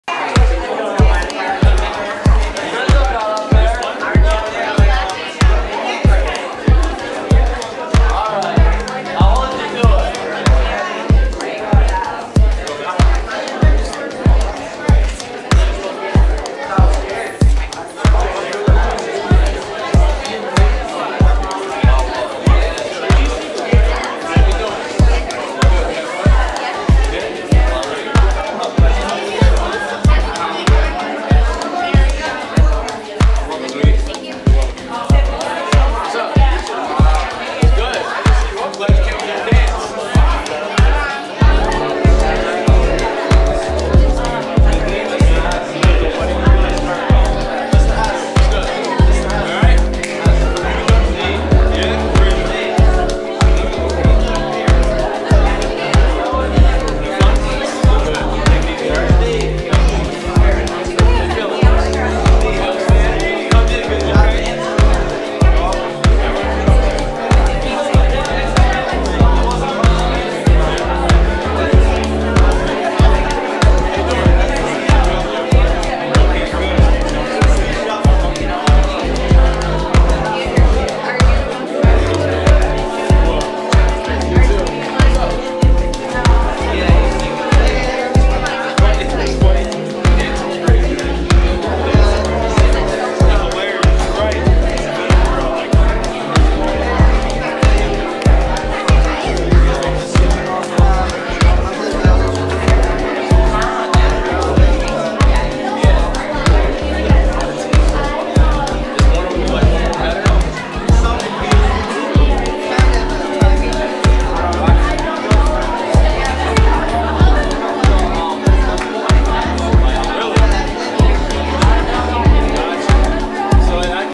JAX DRUMZ “SOUL DRUMMING & YOUTH SPEAKING” MEET & GREET @ ONEIDA-BOCES ON WED 11/2/16
Just a short clip of the meet & greet session on Wed 11/2/16 for students in grades 7-12 at Oneida-Boces located in New Hartford, NY following a "SOUL DRUMMING & YOUTH SPEAKING" event... FYI: I did an event earlier for students in grades K-6 and it was just as exciting & engaging :)))

Facebook fan page: JAX DRUMZ MUZIK PROD